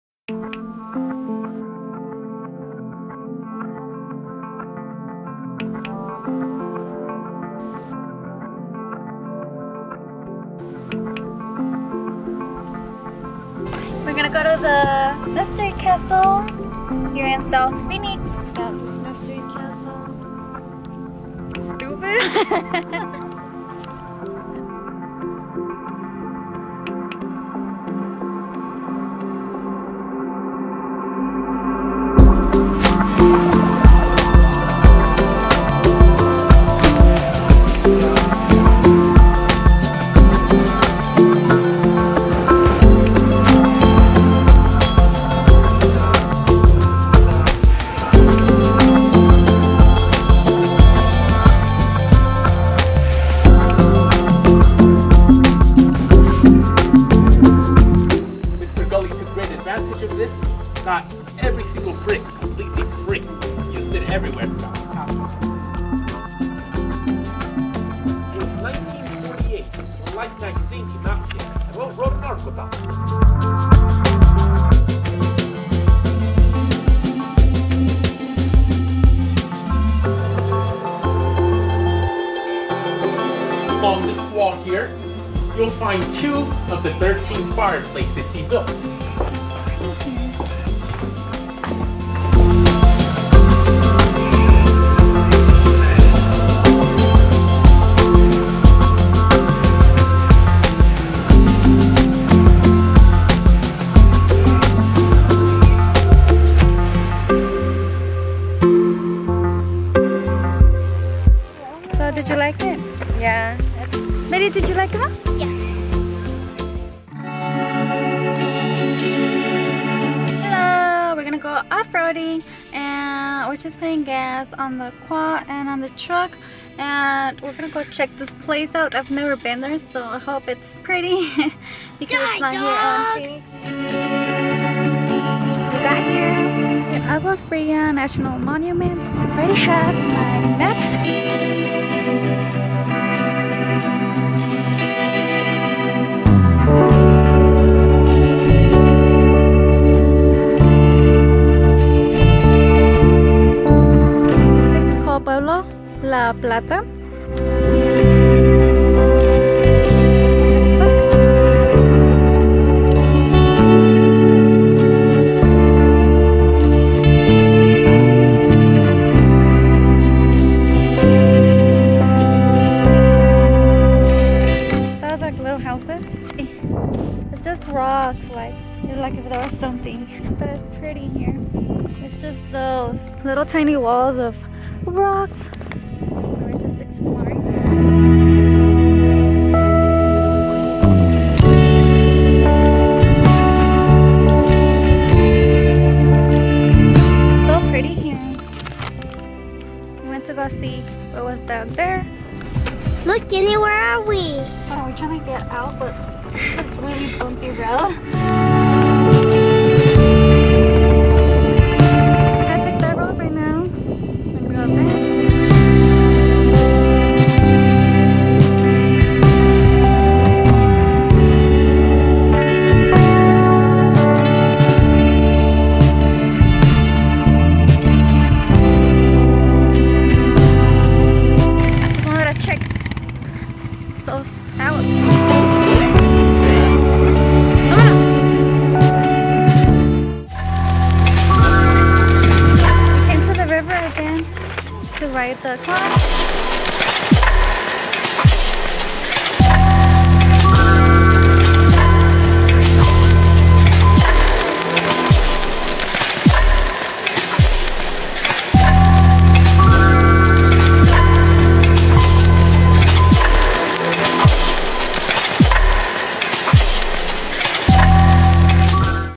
We Visited the Mystery Castle & We Rode the Bloody Basin Road
Hello my beauties!
Welcome back! Here is my monthly Vlog, I didn't do much this month. But some of you dolls wanted me to bring back my Vlogs :)
I know you guys want to see more of my every day life and day to day Vlogs, but I am still very shy and super awkward... very soon Ill bring some! :)
I took a mini tour thru Mystery Castle, a house in South Mountain in South Phoenix. And I went off roading to Bloody Basin Road in Yavapai County and to Salt river in Tolleson, Az.